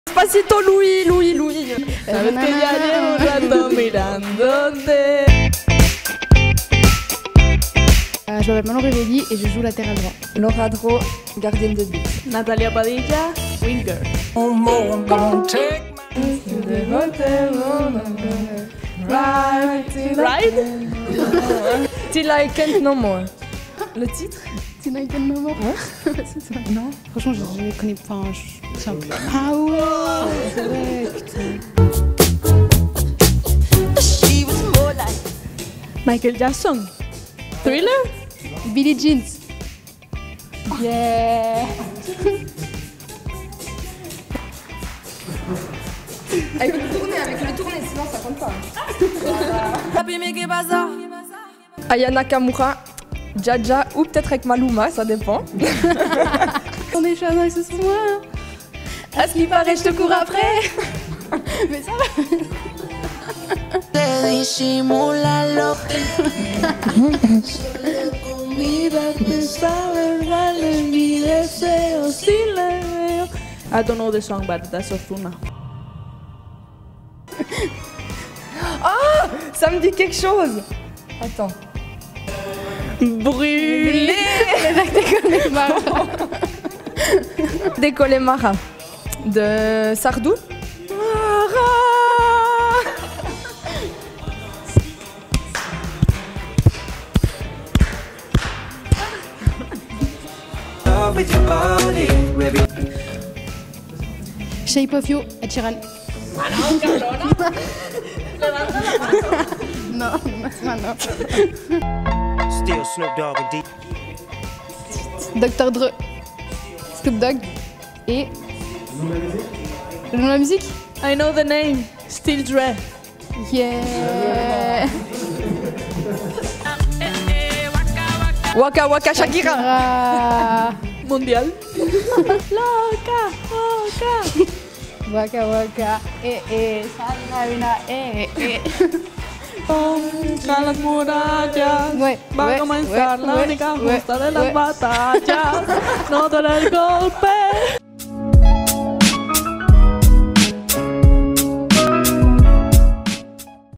Blind Test avec Natalia Padilla, Laura Droz et Manon Revelli 🤣 | SFCCF
Qui sortira vainqueur de ce duel entre Natalia Padilla, Laura Droz et Manon Revelli ?